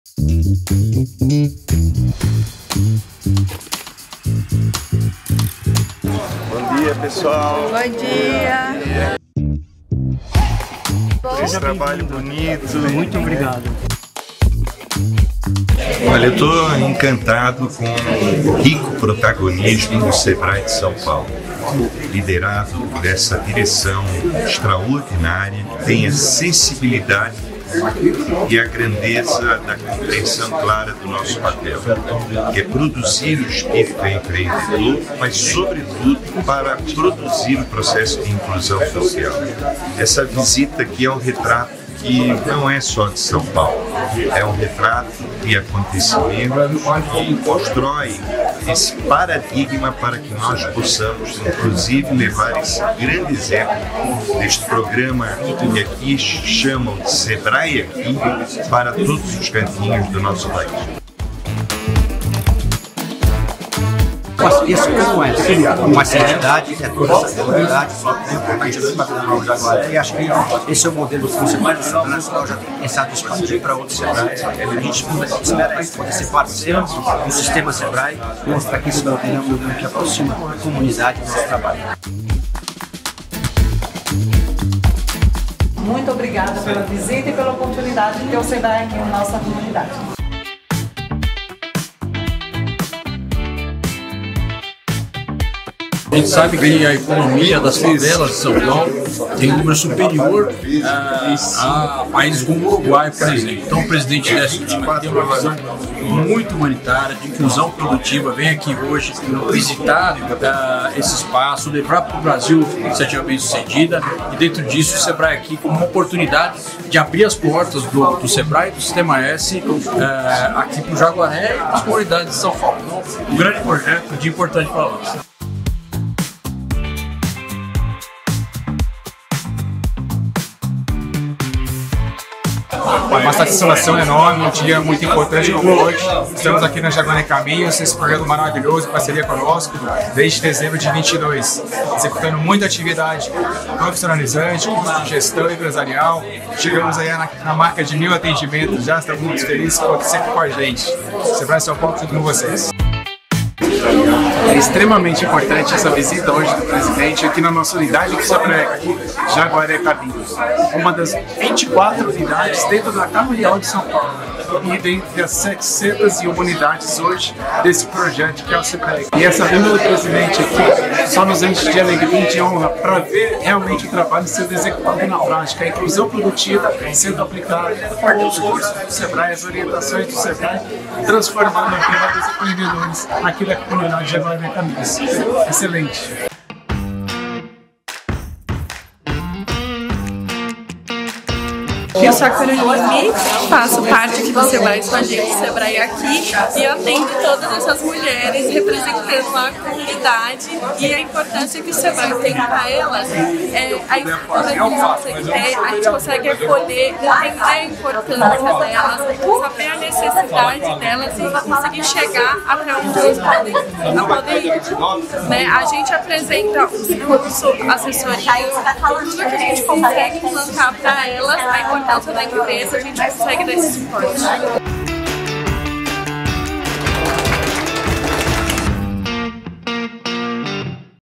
Presidente do Sebrae Nacional e diretoria do Sebrae-SP conhecem programas do Sebrae Aqui Jaguaré.
Lideranças da comunidade Vila Nova Jaguaré, do Sebrae Nacional e do Sebrae-SP estiveram presentes no Sebrae Aqui Jaguaré Caminhos, zona oeste da capital paulista. Na ocasião, o Presidente do Sebrae Nacional, Décio Lima, conheceu o posto de atendimento e alguns clientes da comunidade atendidos no local.